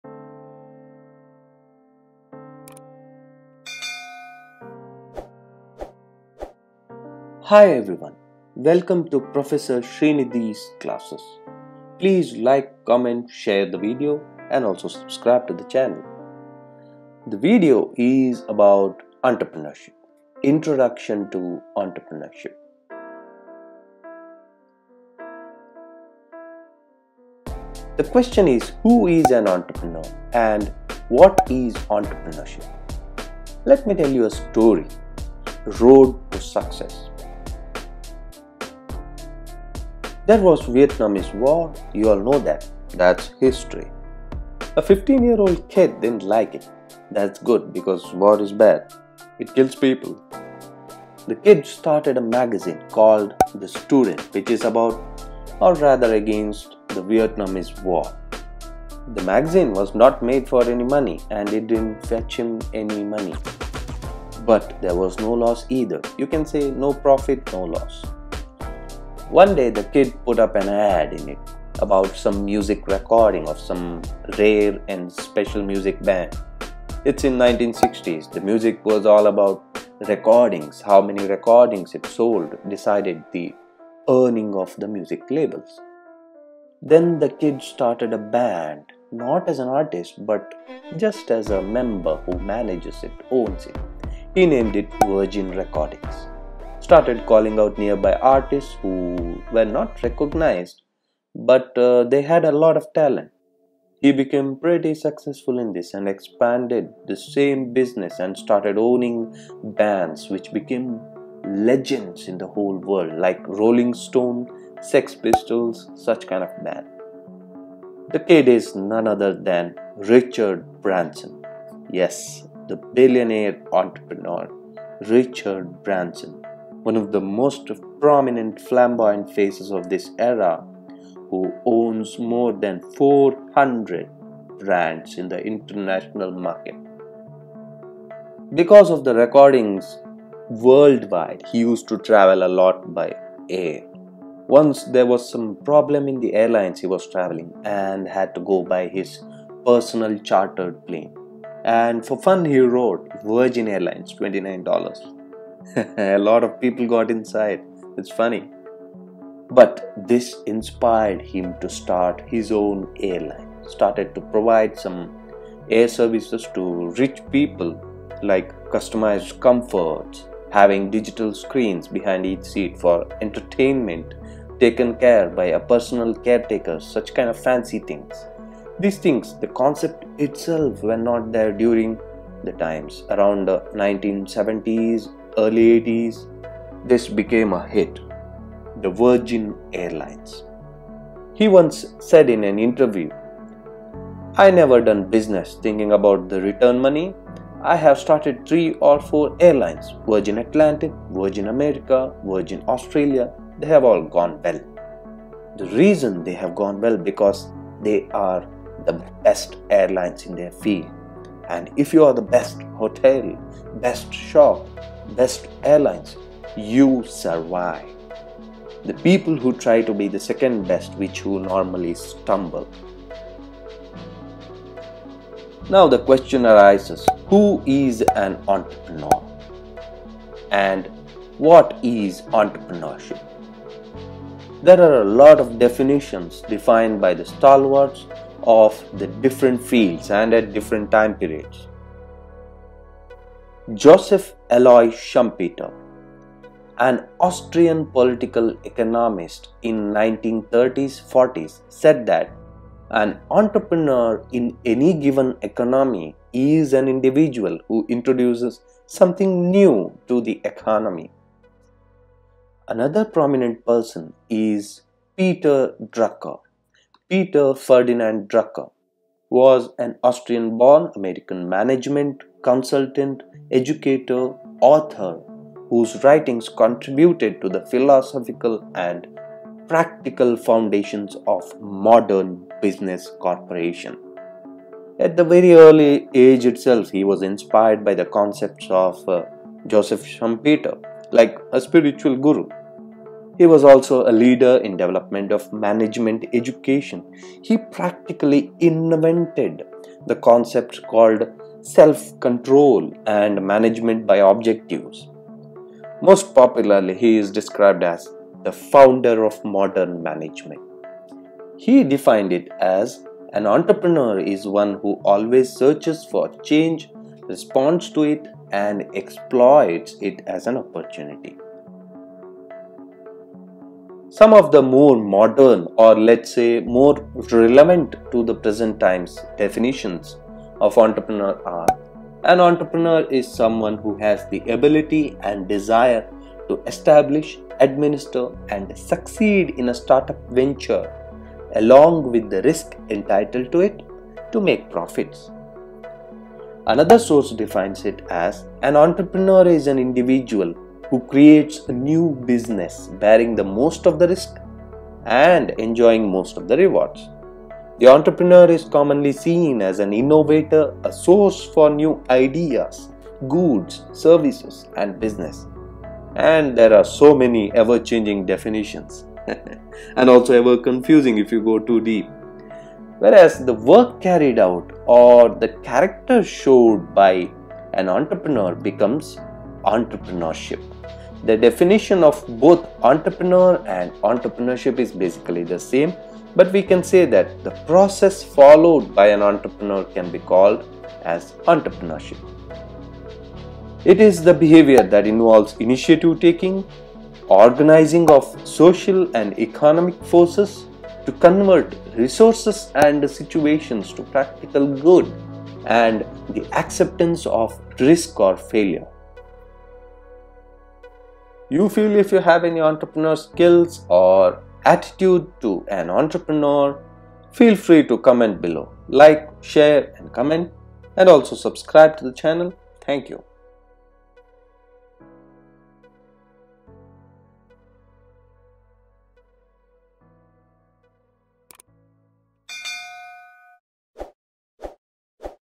Who is an Entrepreneur? | Definition | Road to Success!! Episode 1 | Richard Branson
Online teaching learning classes for engineering enthusiasts of the topic "Entrepreneurship - Definition" under the subject Management & Entrepreneurship.

Topics Covered in the Video:
- Entrepreneur
- Road to Success!! "An Entrepreneur's Story"
- Definitions of Entrepreneur
- Definitions of Entrepreneurship